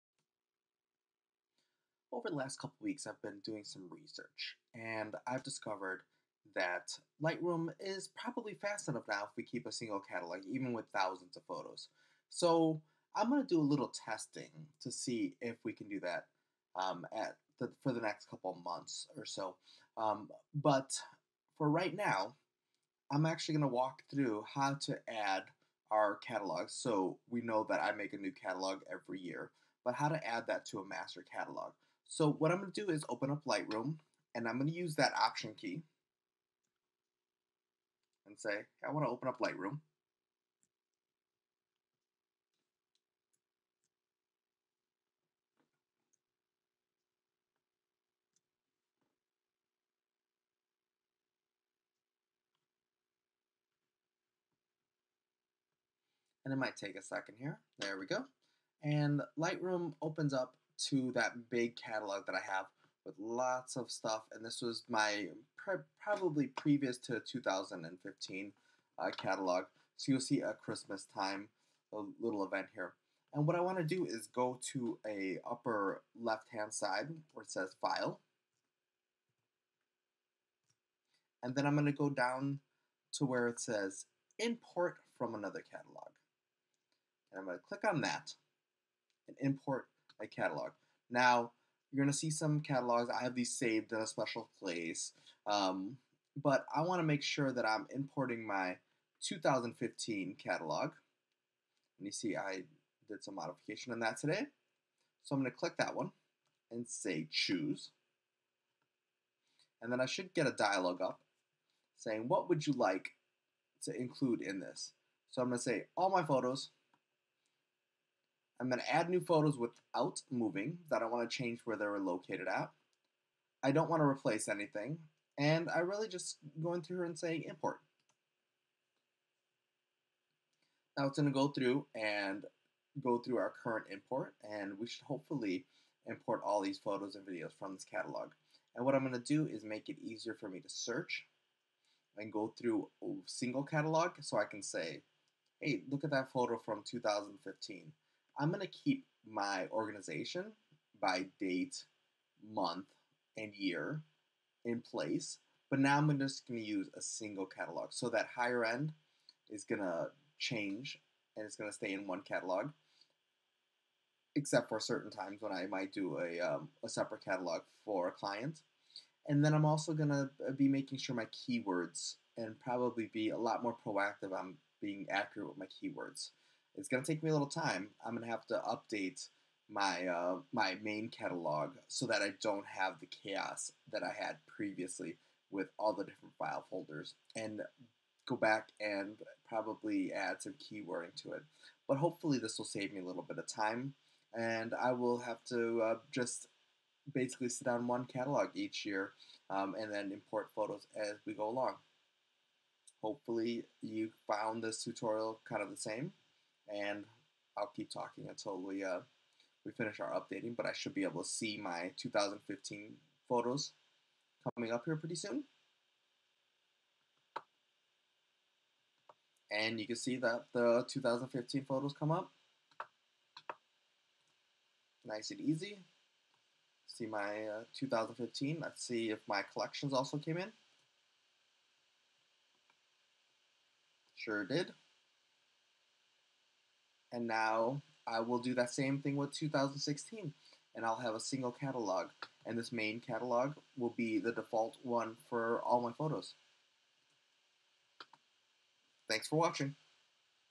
Lightroom Catalog Import
Tutorial on Importing Catalogs. Kinda works back from my class where for the last couple of years I have been saying seperate your catalogs.